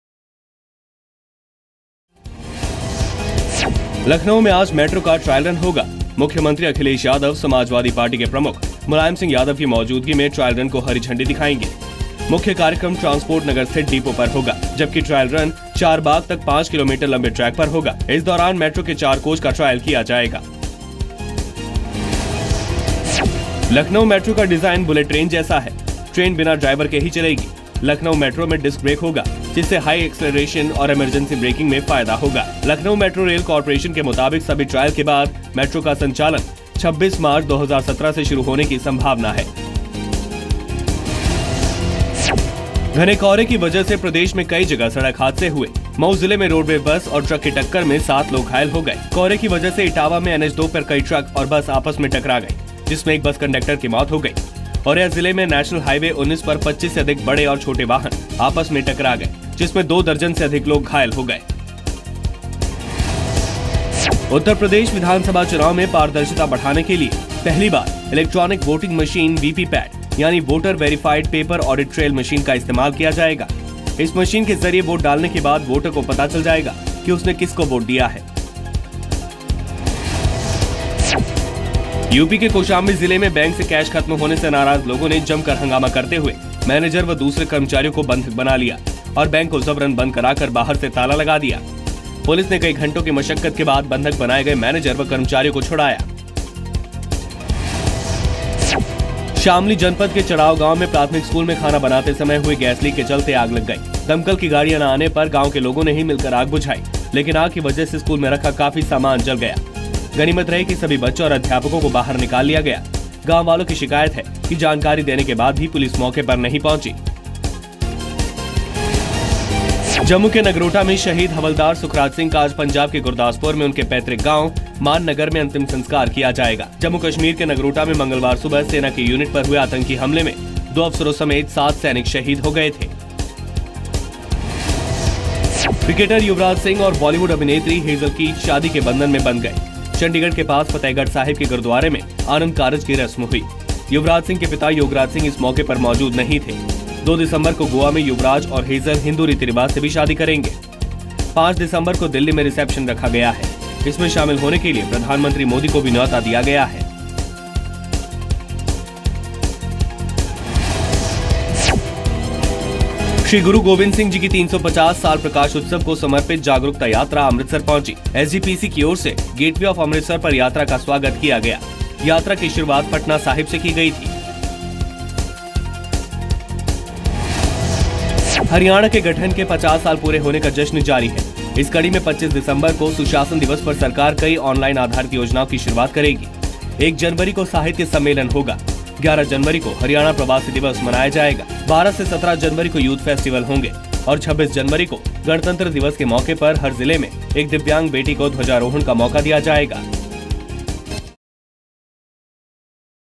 UP PUNJAB TOP 10 | Trail run of Metro to take place in Lucknow today
-Trail run of Metro to take place in Lucknow today
- Lucknow metro to run driverless